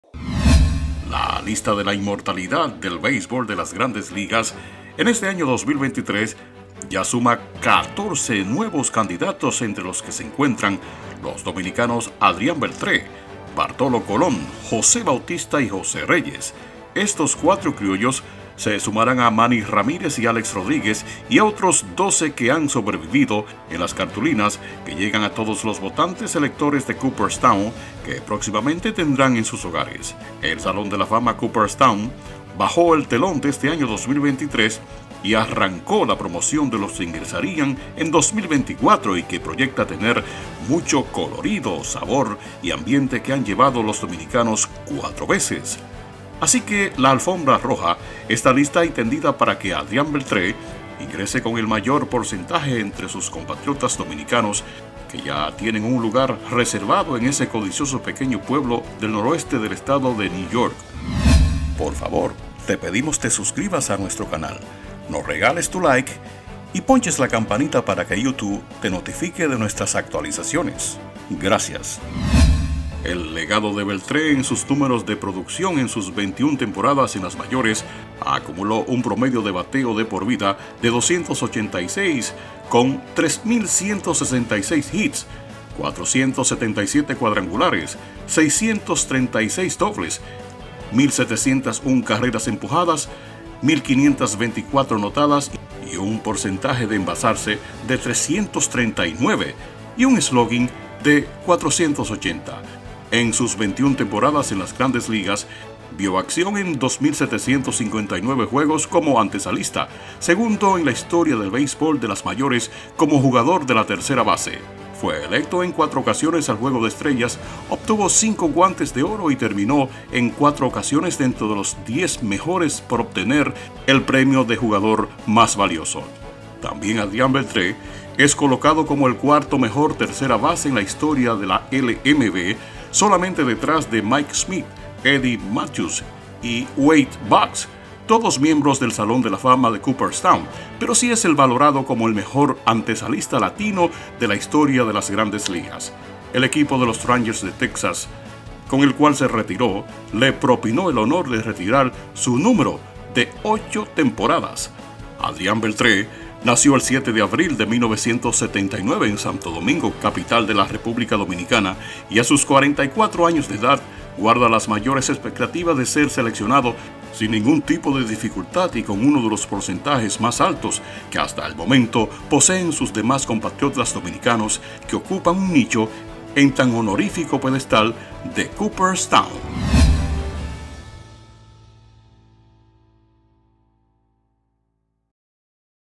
ADRIAN BELTRE, otra estrella DOMINICANA, a COOPERSTOWN
El tercera base Adrian Beltre, que por 21 temporadas vio accion en el beisbol de las grandes ligas, luce indetenible, para ocupar un lugar de honor en el salon de la fama de Cooperstown de los inmortales del beisbol.